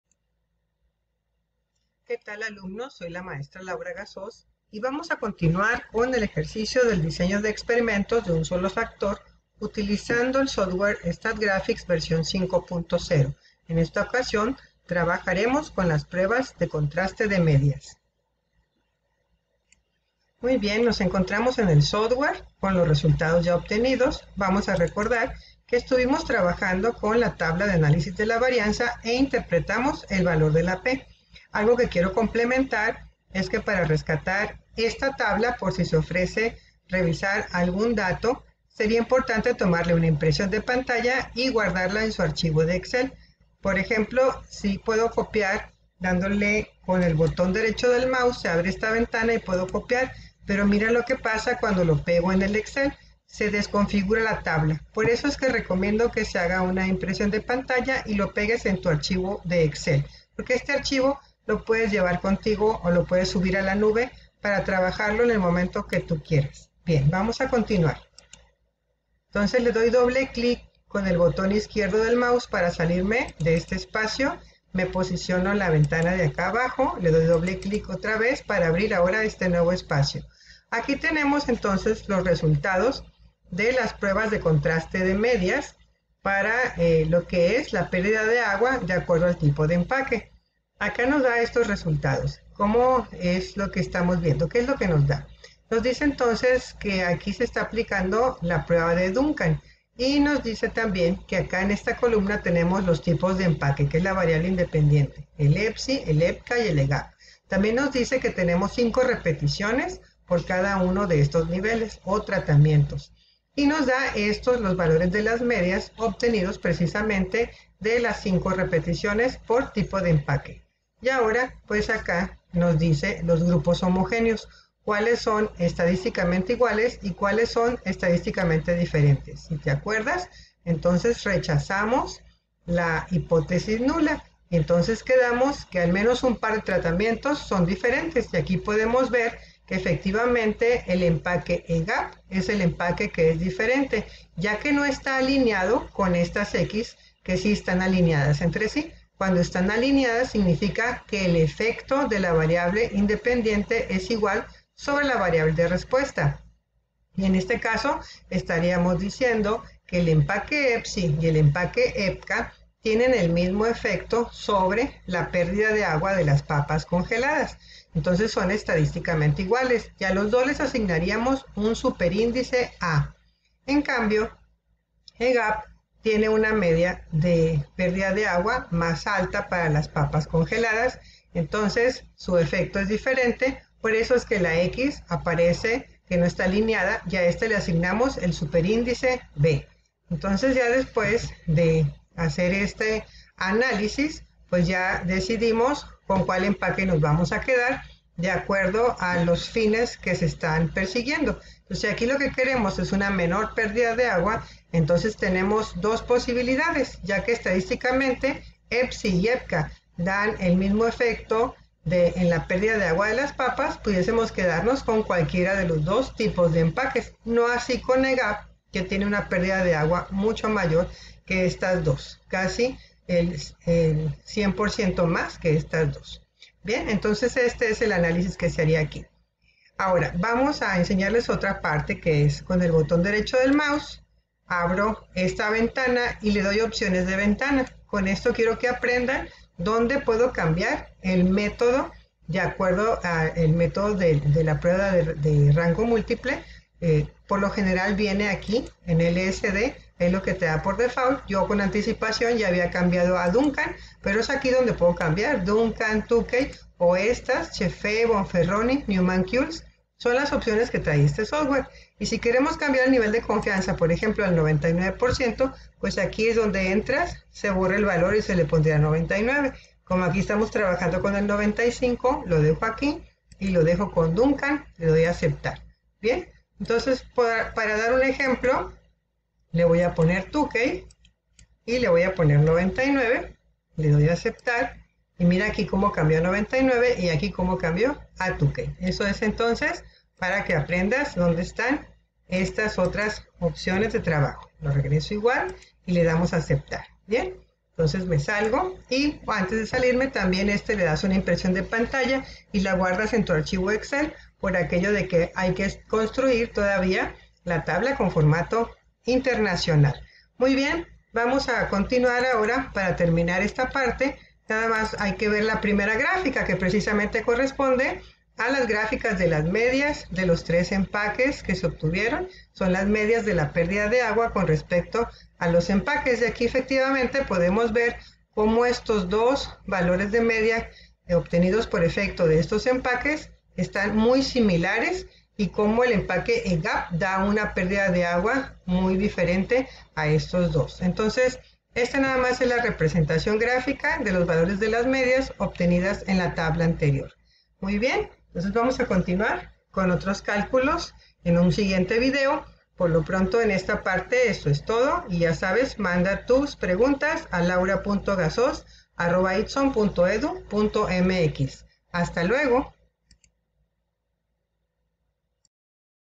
Ejercicio de DOE de un factor Prueba de Contraste de Medias parte 2
Se describen los pasos para obtener los resultados de las pruebas de contraste de medias  con el modelo de un factor (DCA) en el software statgraphics ver 5.0. Es la segunda parte de los cálculos y análisis.